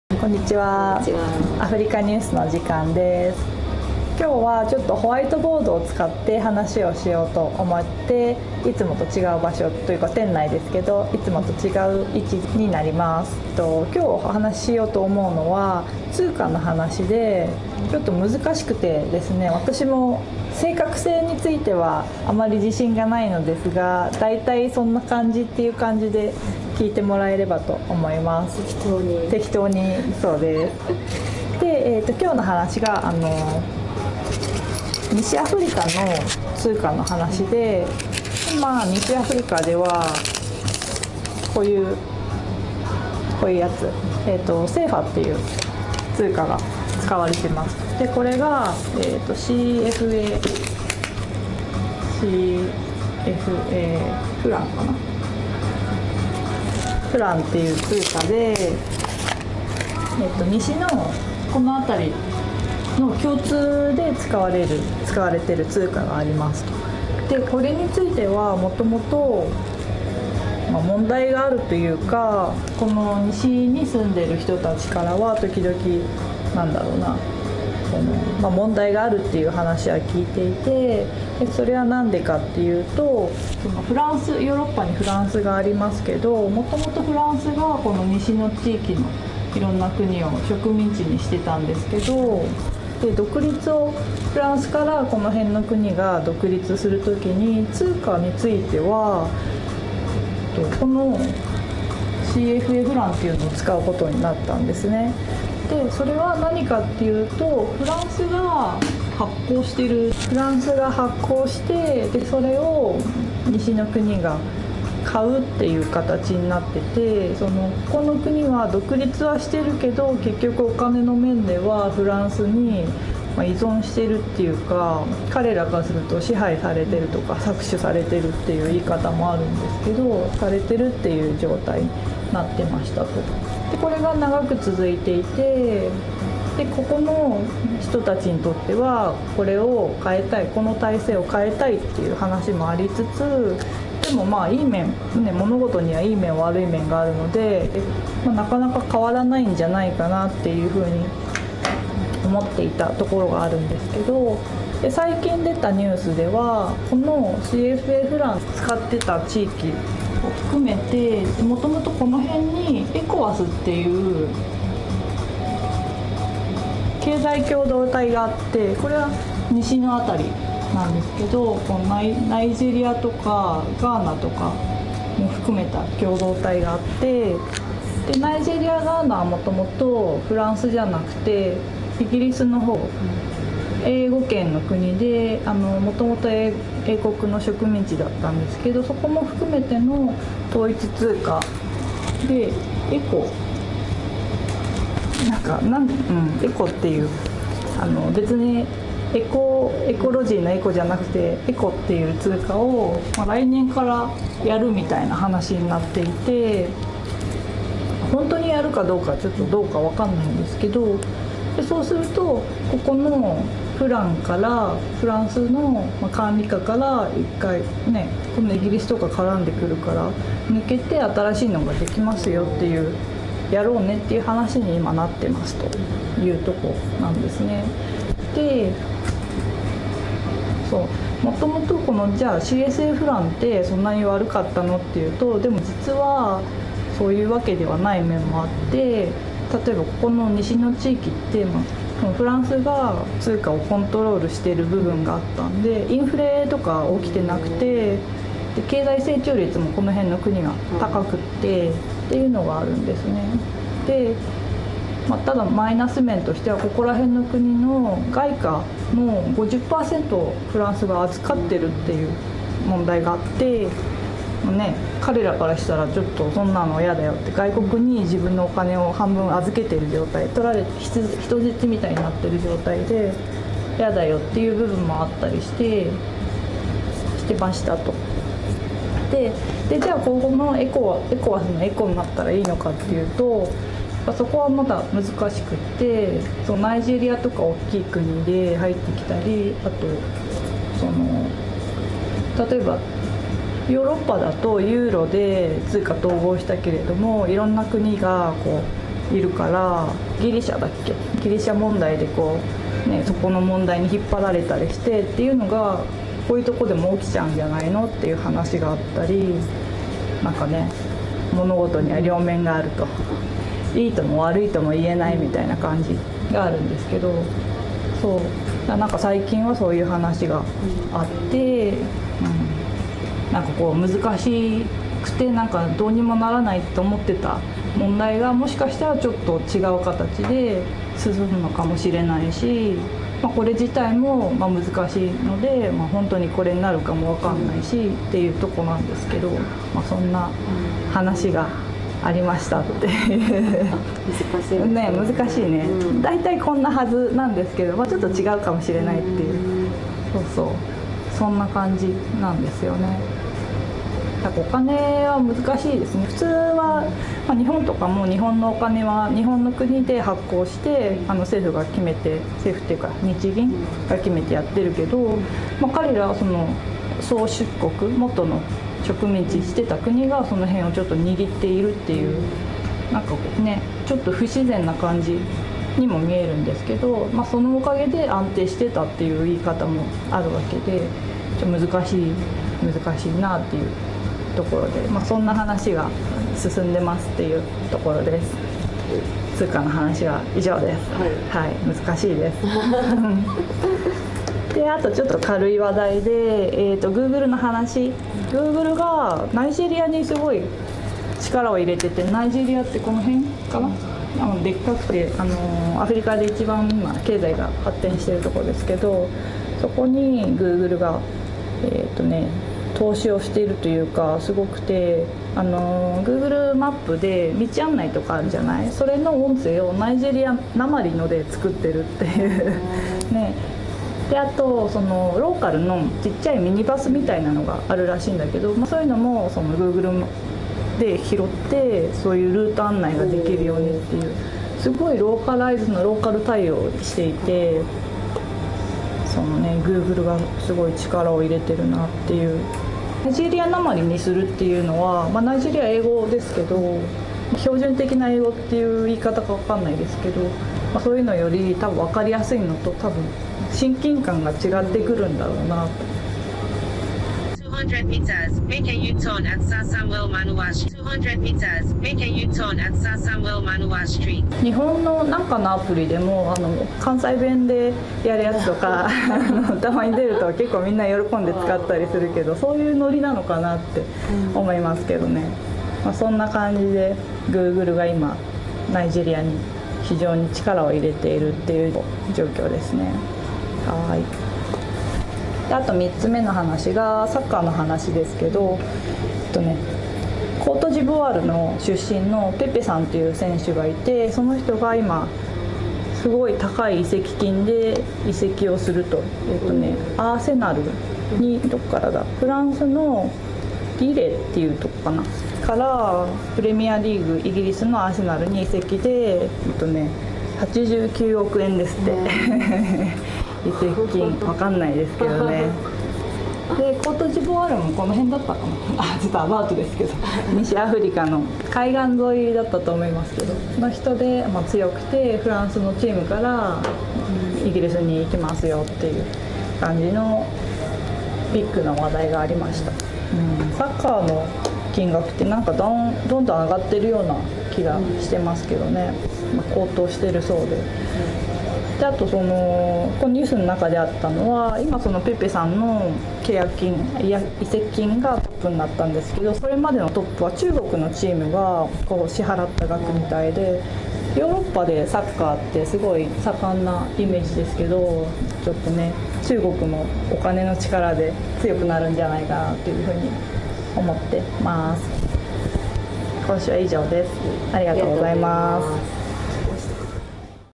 【アフリカ！vol.11】てきとうアフリカニュース AfricaNews11
アフリカのニュースを紹介しています。てきとうだったりもします。

今回はホワイトボードを使ってみました！
てきとうなアフリカの地図書きましたｗ

・難しい西アフリカの統一通貨
・ナイジェリア訛りのGoogle（動画あり！）
・サッカー ペペの高額移籍金
の3本をお送りします。

◆字幕編集はVrewという無料ソフトでやっています。便利！

◆ニュースの元ネタはこちら
「2019年8月1週目のアフリカ最新ニュース」

Shot & edited by Jiyoung-san